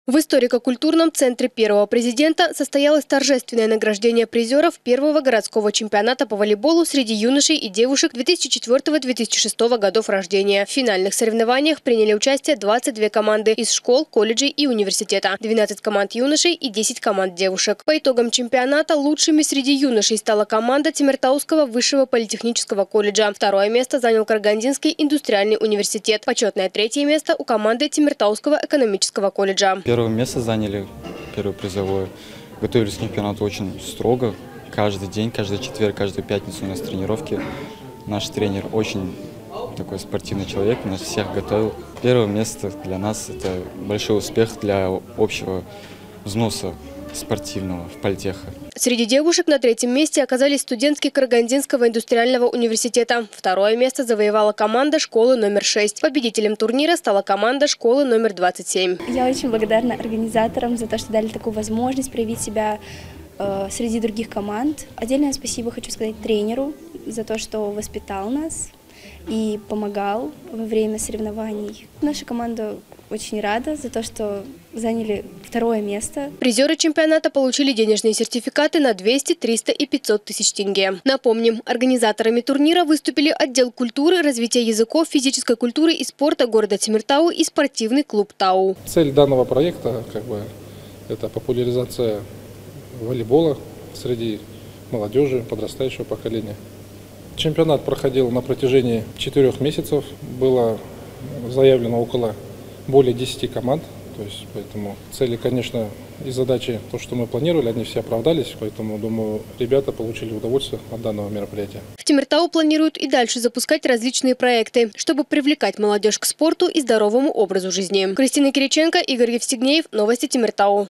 Наградили лучших
В Темиртау подвели итоги первого городского чемпионата по волейболу среди молодёжи.

В историко-культурном центре Первого Президента состоялось торжественное награждение призёров первого городского чемпионата по волейболу среди юношей и девушек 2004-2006 годов рождения. В финальных соревнованиях приняли участие 22 команды из школ, колледжей и университета: 12 команд юношей и 10 команд девушек.

По итогам чемпионата лучшими среди юношей стала команда Темиртауского высшего политехнического колледжа. Второе место занял Карагандинский индустриальный университет. Почётное третье место у команды Темиртауского экономического колледжа.

Темирлан Аманкельдиев, участник команды ТВПК: «Первое место заняли. Готовились к чемпионату строго, каждые четверг-пятницу тренировки были. Наш тренер спортивный человек, он нас всех готовил. Первое место для нас – большой успех, для общего спортивного взноса политеха».

Среди девушек на третьем месте оказались студентки Карагандинского индустриального университета. Второе место завоевала команда школы № 6. Победителем турнира стала команда школы № 27.

Раяна Амантаева, участница команды ОШ № 6: «Я очень благодарна организаторам чемпионата за такую возможность проявить себя среди других команд. Отдельное спасибо тренеру за то, что воспитал нас и помогал во время соревнований. Наша команда очень рада, что заняли второе место».

Призёры чемпионата получили денежные сертификаты на 200, 300 и 500 тысяч тенге.

Напомним, организаторами турнира выступили Отдел культуры, развития языков, физической культуры и спорта города Темиртау и спортивный клуб «Тау».

Алмат Серимов, руководитель спортивного клуба «Тау»: «Цель данного проекта – популяризация волейбола среди молодёжи, подрастающего поколения. Чемпионат проходил на протяжении четырёх месяцев. Было заявлено более 10 команд. Поэтому цели и задачи, которые мы планировали, все оправдались. Думаю, и ребята получили удовольствие от данного мероприятия».

В Темиртау планируют и дальше запускать различные проекты, чтобы привлекать молодёжь к спорту и здоровому образу жизни.